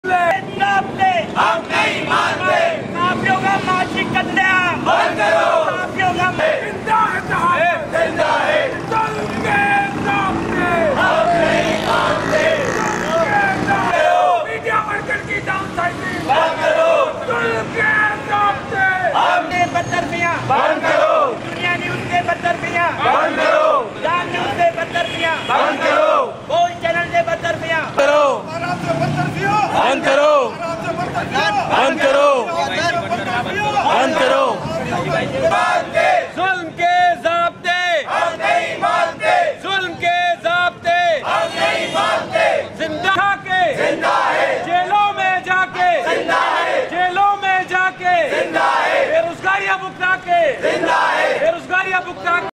Journalist protest outside Hyderabad Press Club against forced forced journalists from TV channels a
ملک بھر میں ٹی وی چینلز اور اخبارات سے صحافیوں کی جبری برطرفی کے خلاف حیدرآباد پریس کلب کے باہر صحافی برادری کا احتجاجی مظاہرہ